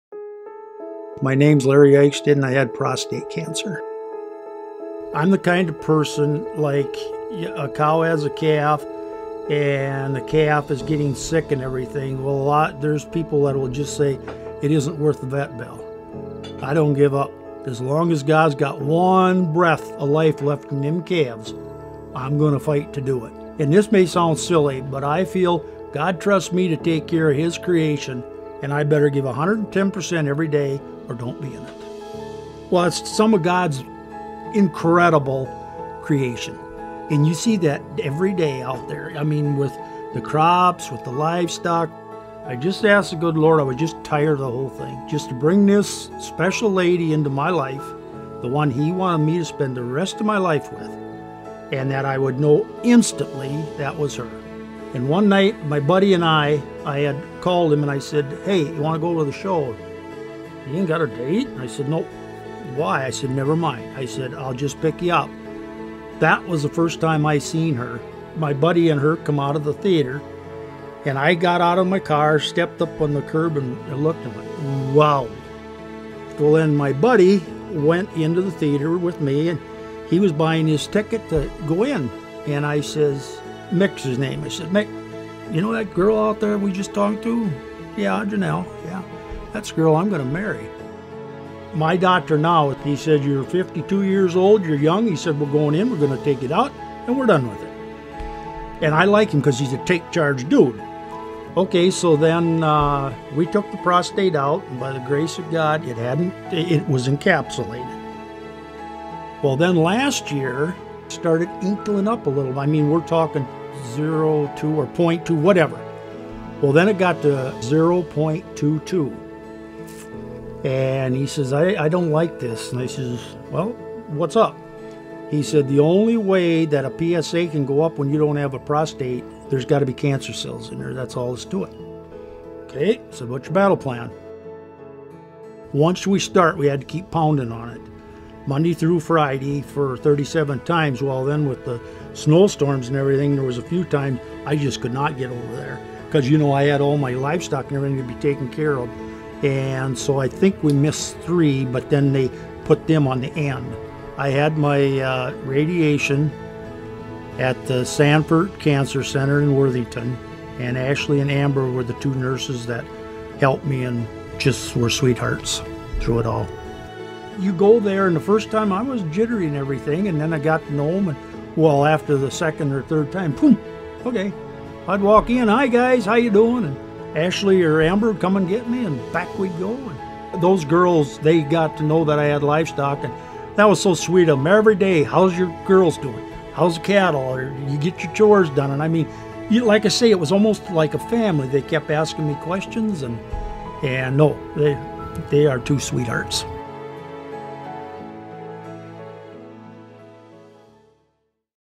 Cancer Survivor Shares the Value in Regular Screenings
After getting his prostate removed, Larry Eichstadt continued regular screening. This persistence helped Larry’s doctor catch his cancer early.